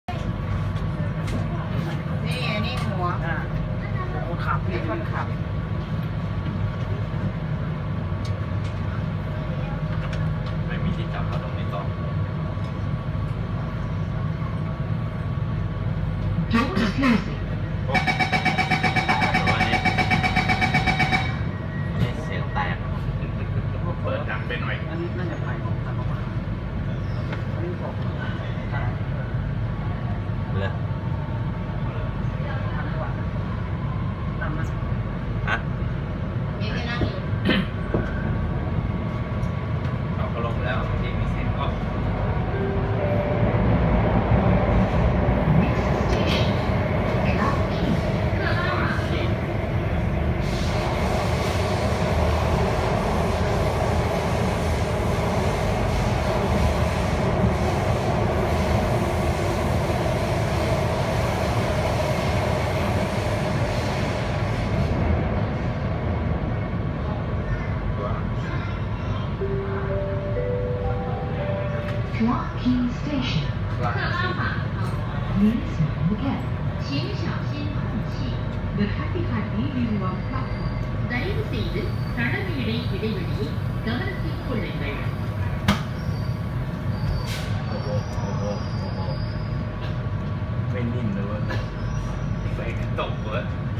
SBS Transit Alstom Metropolis C751C Ride: NE4 Chinatown to NE5 Clark Quay
SBS Transit new North East Line train Alstom Metropolis C751C from NE4 Chinatown to NE5 Clark Quay. Bound for Punggol